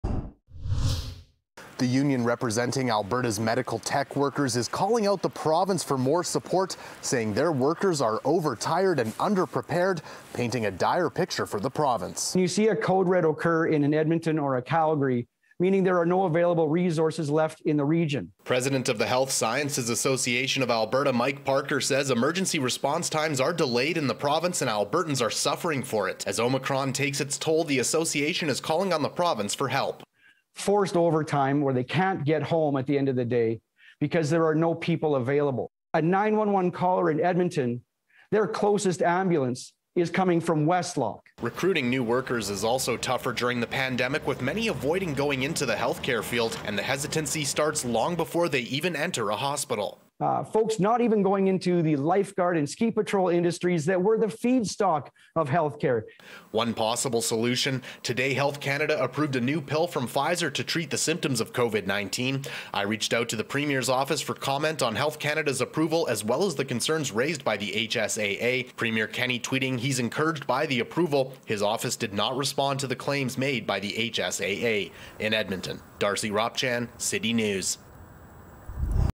HSAA says Alberta is in a code red
The president of the Health Sciences Association of Alberta telling media Monday the province’s first responders are overworked and underprepared.